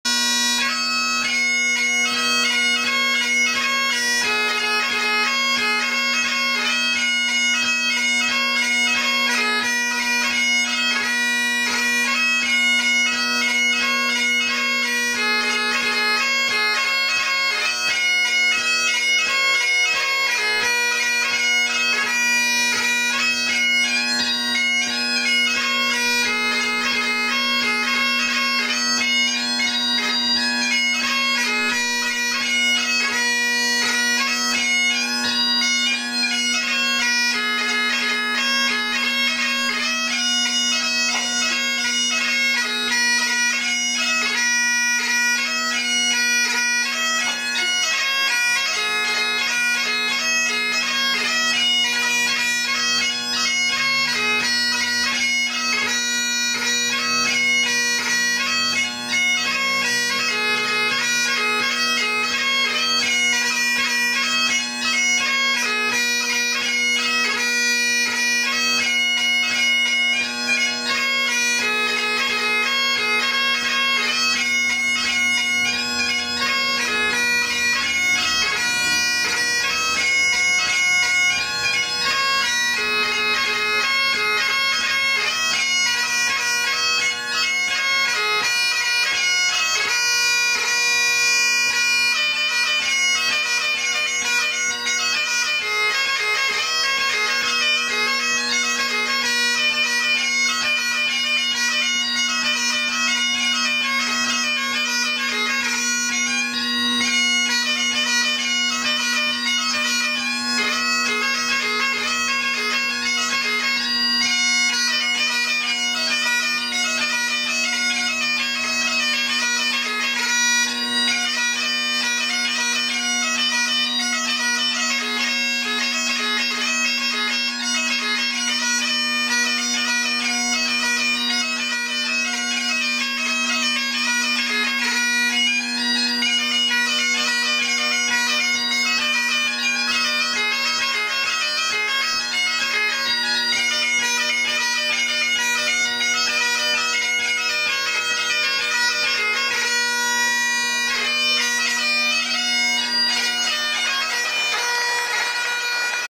Craig Sutherland: Hornpipe & Jig - Pipe Idol 2012
Craig Sutherland plays his hornpipe and jig for the 2012 Pipe Idol contest, as part of Glasgow's "Piping Live 2012" week.